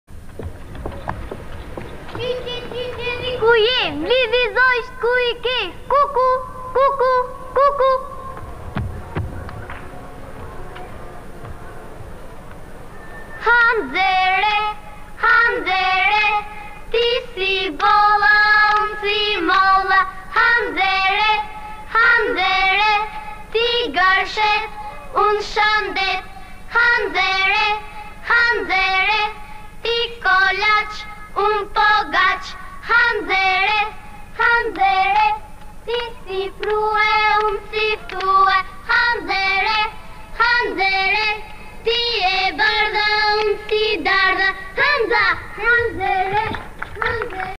Hanëz e re
Rituali i hanës (hanzës) së re që shfaqet në fund a në fillim të muajit. Jo më kot fëmijët i thonë hanëz (hënëz) sepse ajo s'është e plotë dhe ato e krahasojnë me bollën e me prroin ndërsa veten e tyre me mollën e me ftoin që janë të plotë e me shëndet.
Vajzat veshin veshje të ndryshme të Lumës, Kukës ndër to veshja me kanac.
Festivali Foklorik Kombëtar Gjirokastër, 1983.